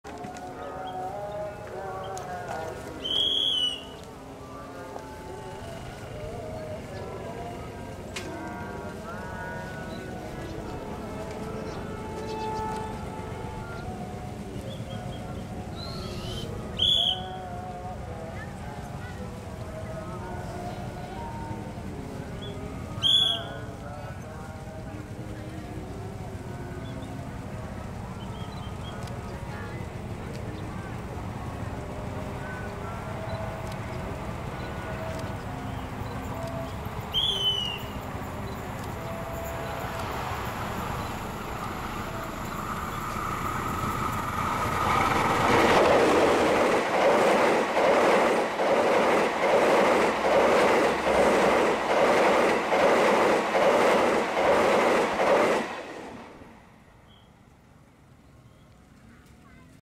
Kereta Api Ekonomi Baru atau K3 2016 New Livery Rute Surabaya Gubeng - Pasar Senen
Kereta Api Ekonomi Baru buatan INKA 2016 melintasi daerah sepanjang pada tanggal 28 Agustus 2016 dengan Rute Surabaya Gubeng - Pasar Senen yang rencanaya menggantikan beberapa kereta Kelas Bisnis